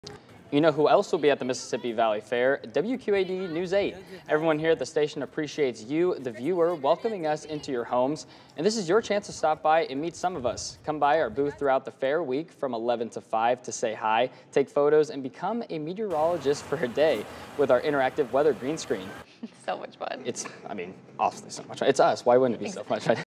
Meet WQAD at the Mississippi Valley Fair!
Our booth will feature our on-air talent and will give kids a chance to be a meteorologist for a day with our interactive green screen.